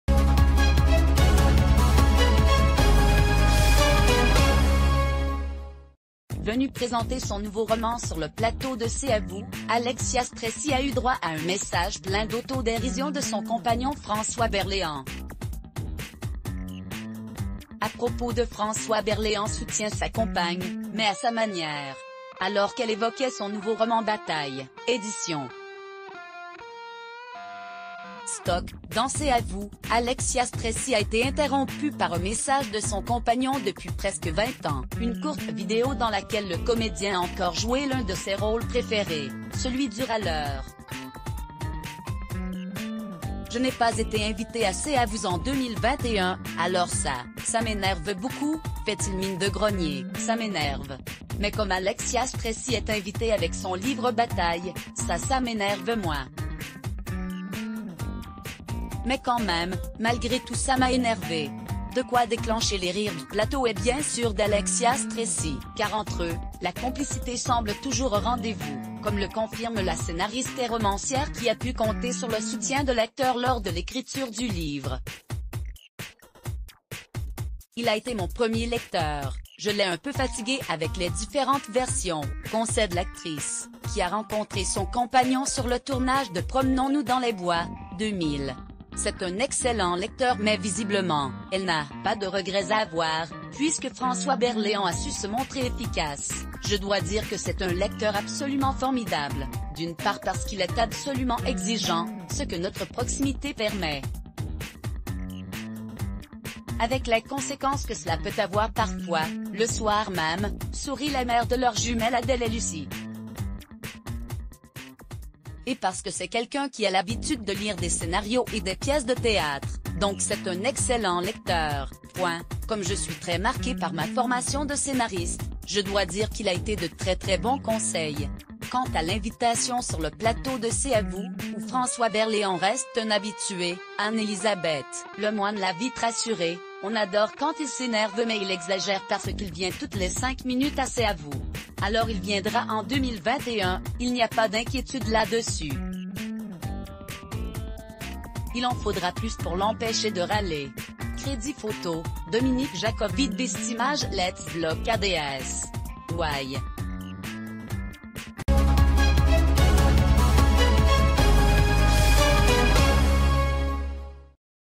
VIDÉO – « Ça m'énerve ! »: le message ironique de François Berléand à sa compagne Alexia Stresi
Venue présenter son nouveau ro...